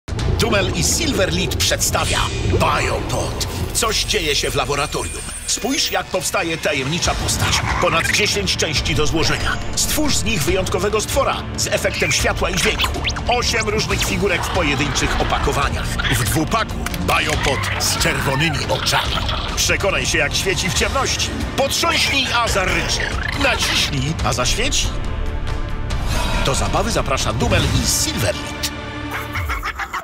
BioPod Dumel Silverlit
Otwórz tajemną kapsułę i wywołaj do zabawy magiczne stwory! W środku pudełka znajdziesz elementy, z których stworzysz aż dwie postaci. Dinozaur posiada wyjątkowe zdolności, przy potrząsaniu wydaje realistyczne odgłosy! Dodatkowo, po naciśnięciu na przycisk, znajdujący się na jego głowie, wyda on dźwięk, a jego oczy rozbłysną. Jeden ze stworków potrafi też świecić w ciemności! Wystarczy, że wystawisz go na światło słoneczne lub sztuczne przez około 2 minuty, a następnie umieścisz go w ciemnym miejscu.

Produkt przeznaczony jest dla dzieci od 5 roku życia.